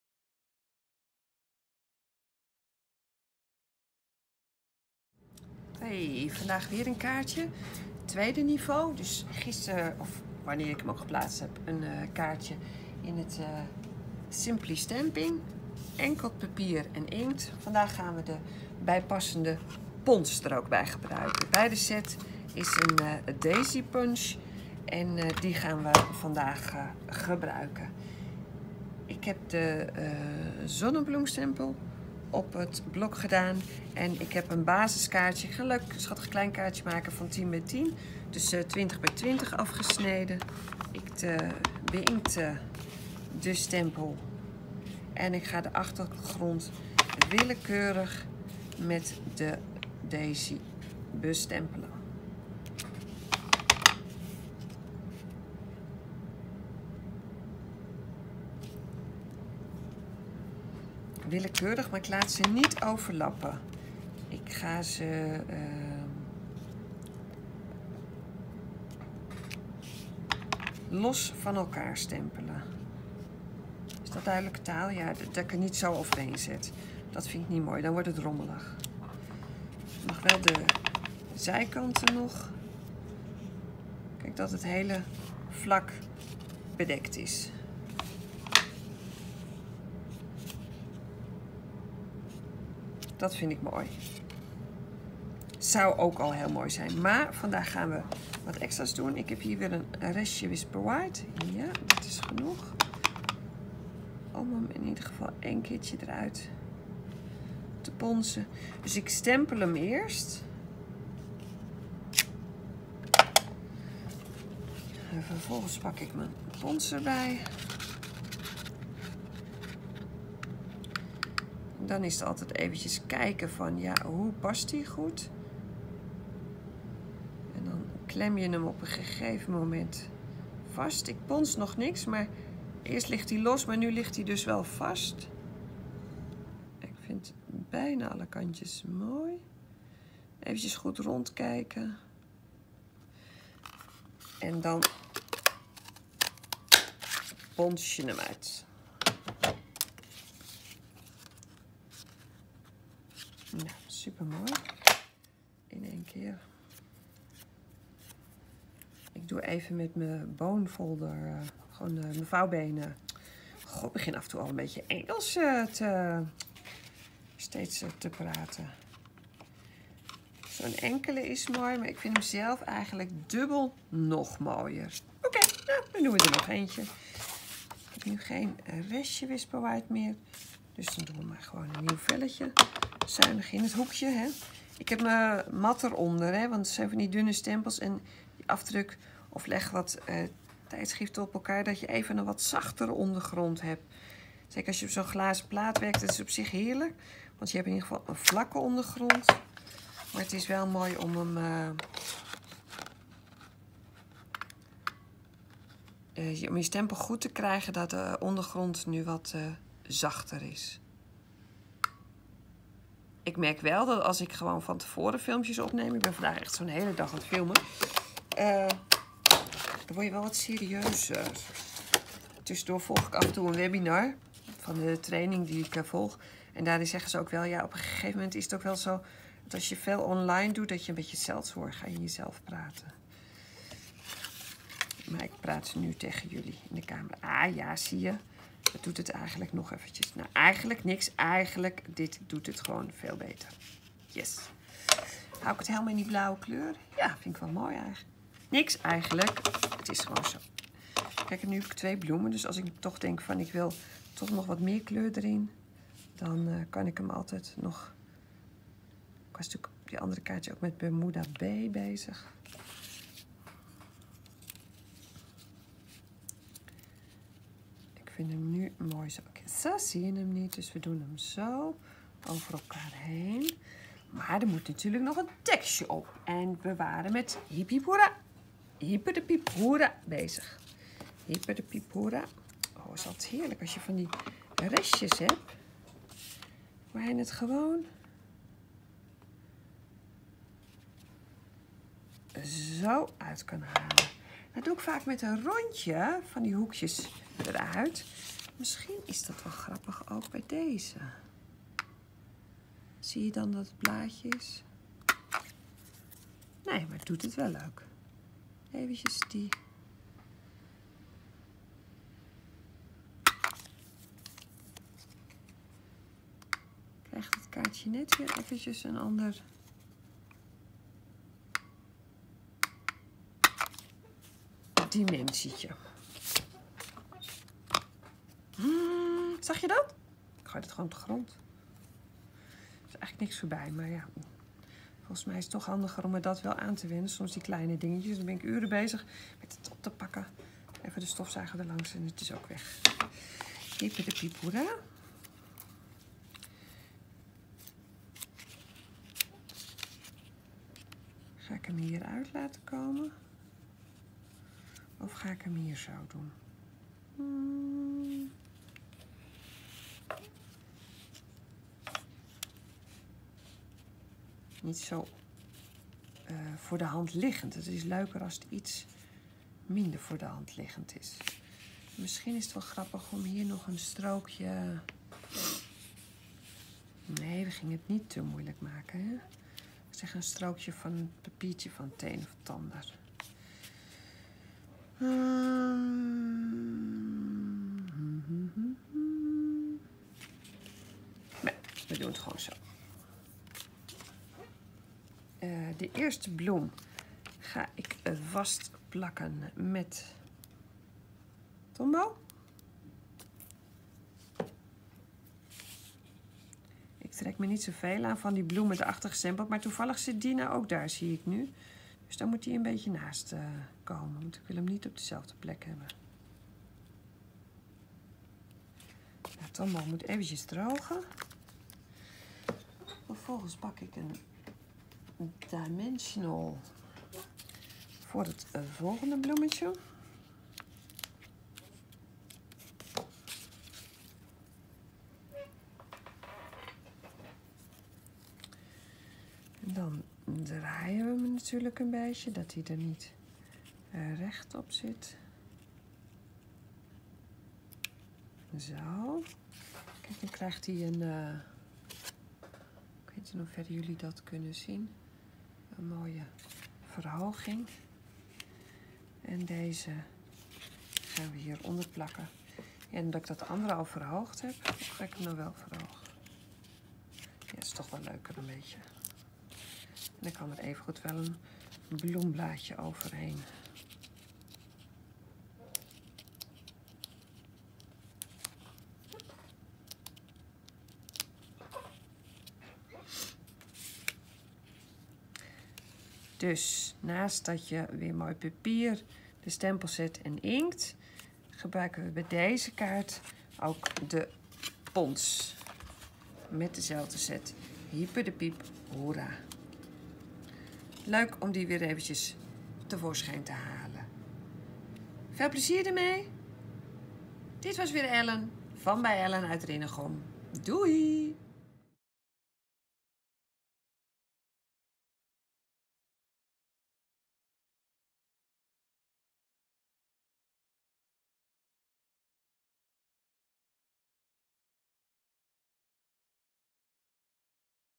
Daisy delight deel 2 met geponsde bloem. Nl
De bijpassende pons maakt de set helemaal super compleet. Hier zijn twee bloemen over elkaar verwerkt en geven de kaart een chique frisse uitstraling.
De tekst is uit een SAB set die alleen tijdens de SAB voorjaar 2019 te verdienen is bij een aankoop van 60 euro. Informeer graag naar de mogelijkheden.